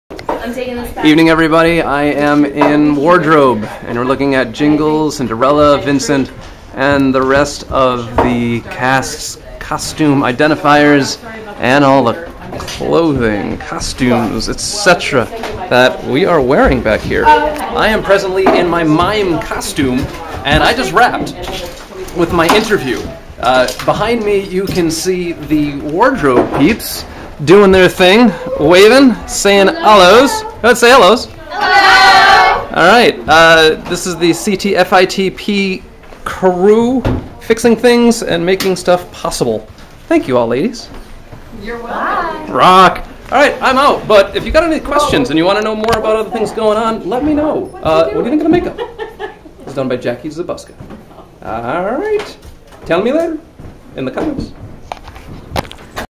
On-set Day 3 -- A-1 Quality Entertainment
-- I'll be shooting short videos highlighting key things on-set at "A-1 Quality Entertainment" during the CT FITP shoot.

In this video meet our awesome costume department also mime make-up by Jackie Zbuska.

What would you like to see/hear? Let me know below.